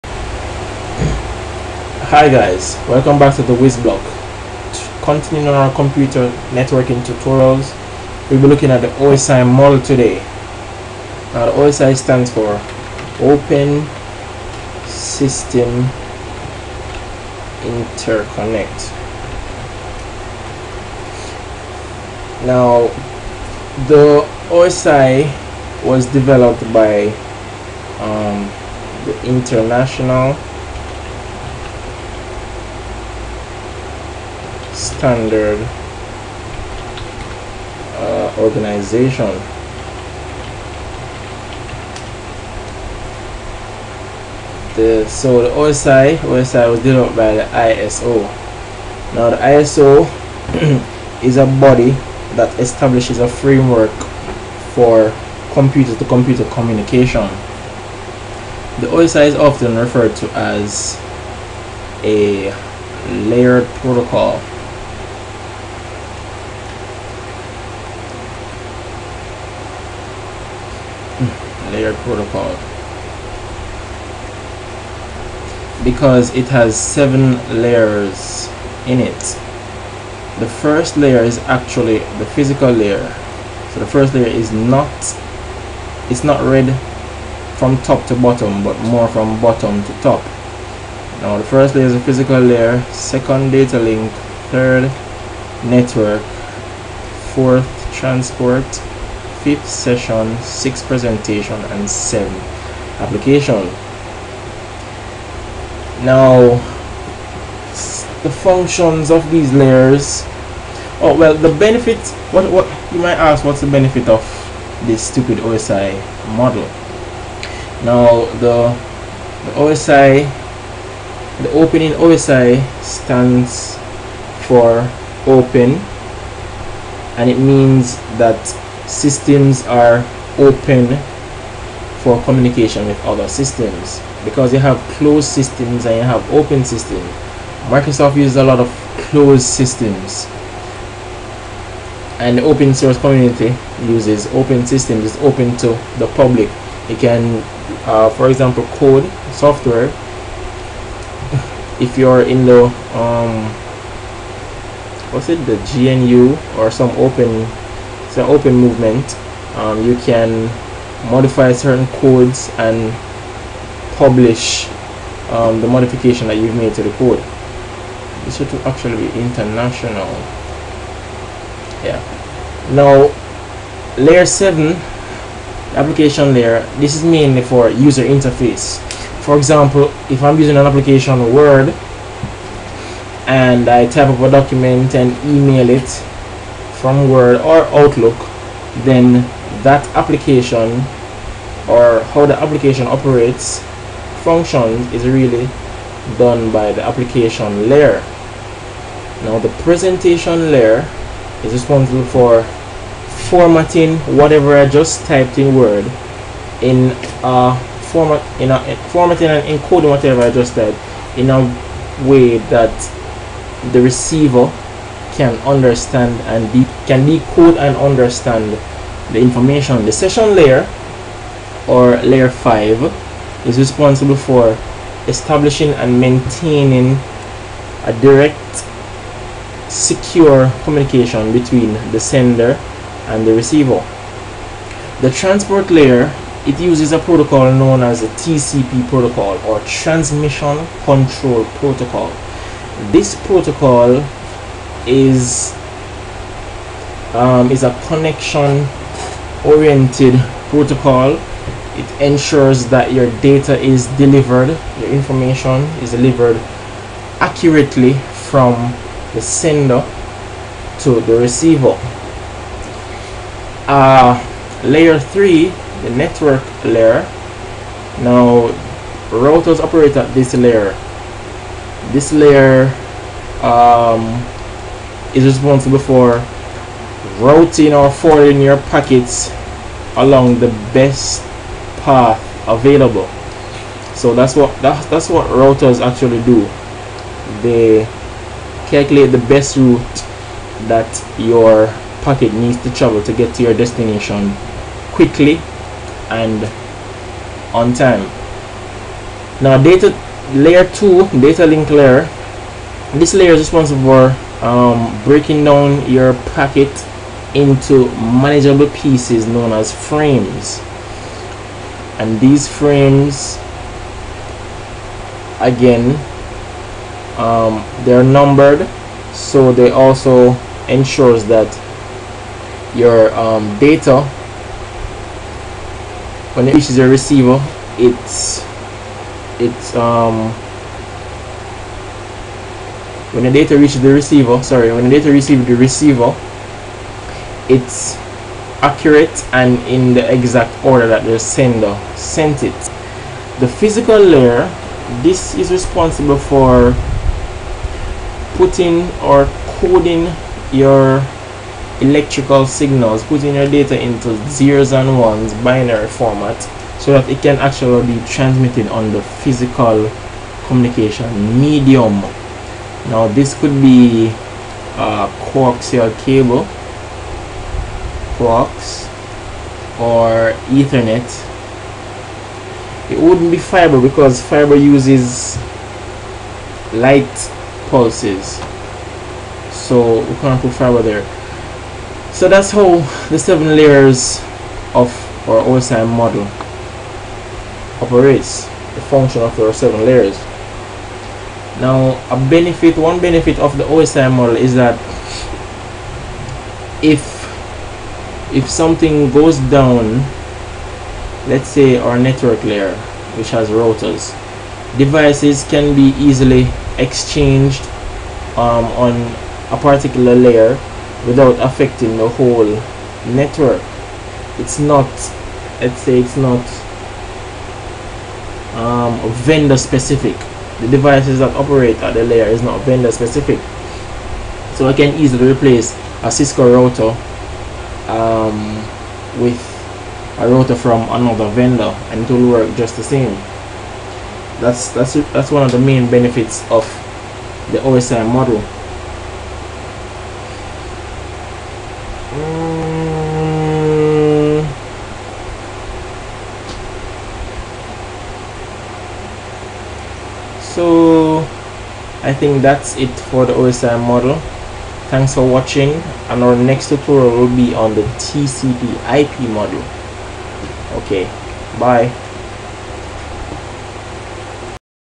Computer Networks Tutorial - 4 - OSI Model
Explanation of the OSI model and the layers that operate in the model.